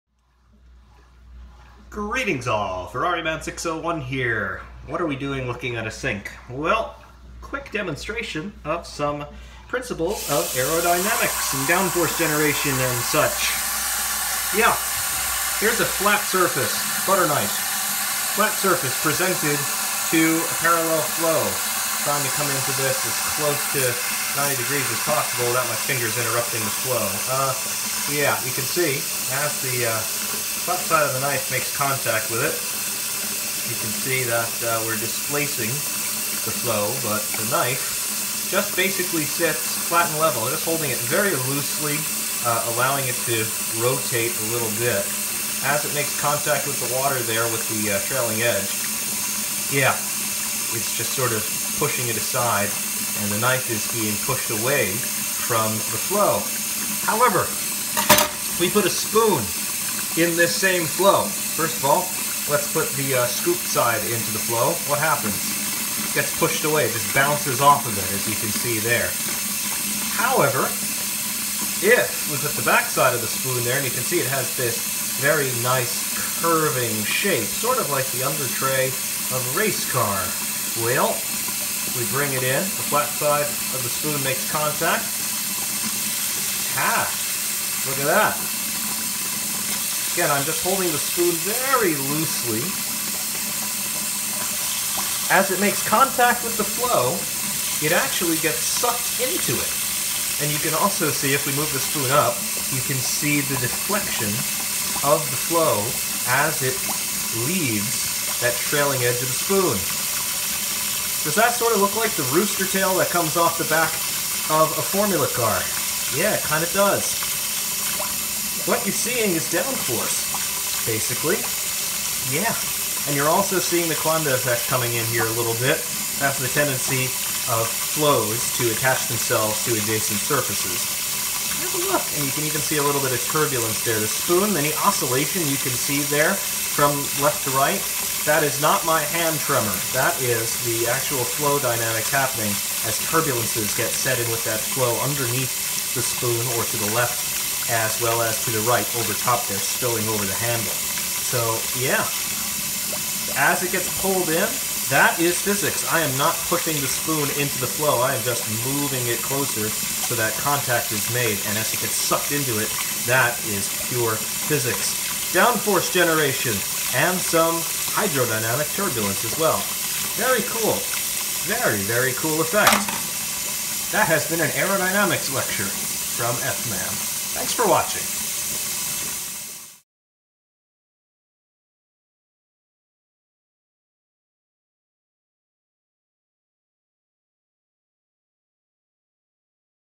Making Downforce at Home - Try It!
A simple demonstration displaying the Coanda effect and rudimentary downforce generation. Headphone users might want to take necessary precautions.